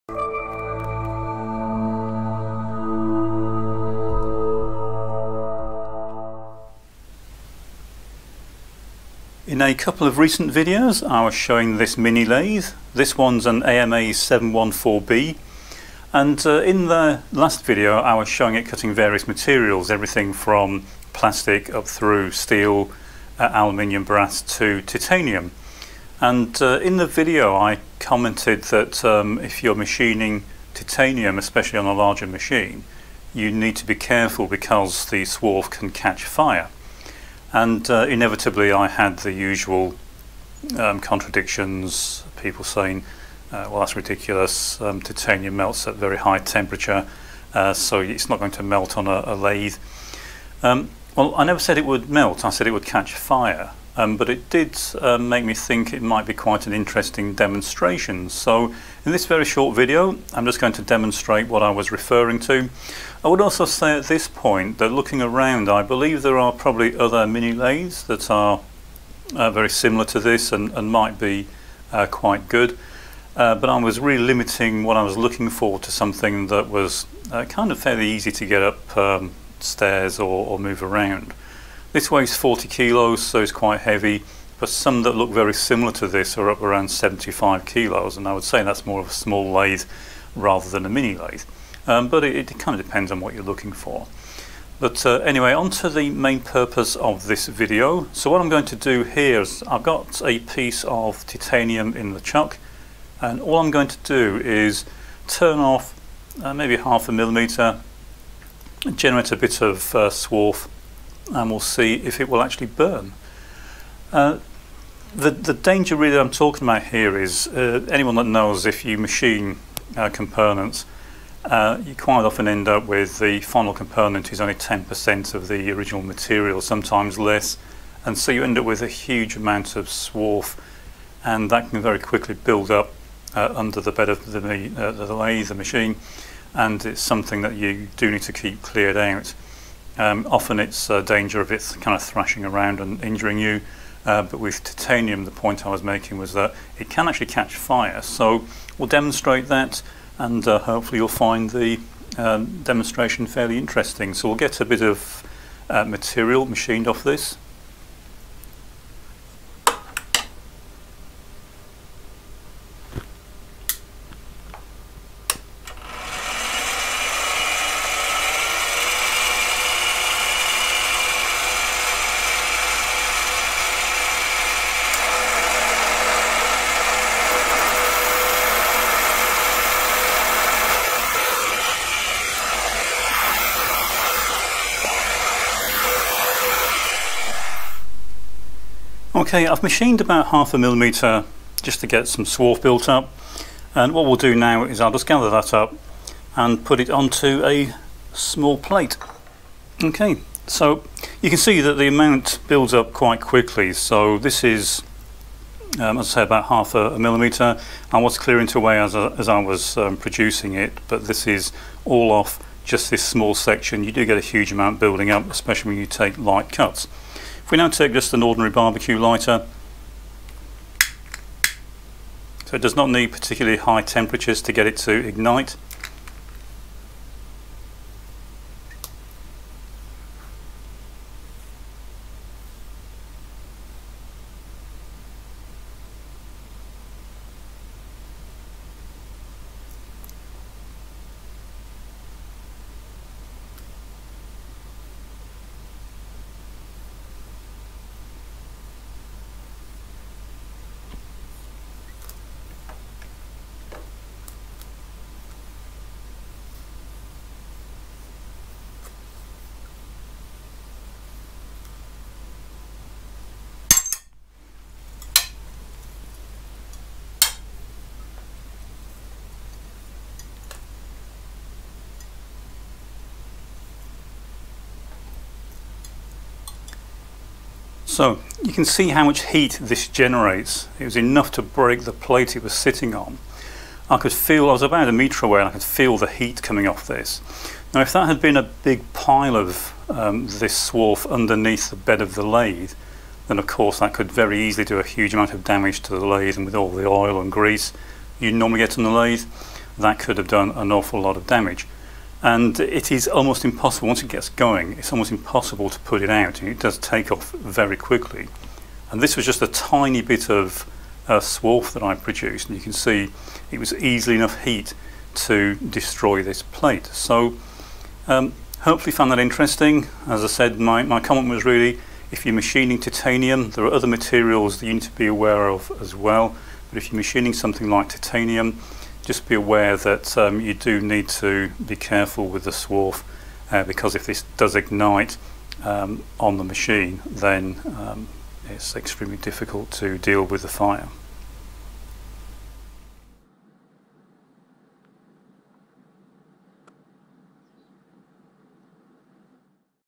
Burning Titanium and breaking stuff
In this short video I set fire to my lathe and break something.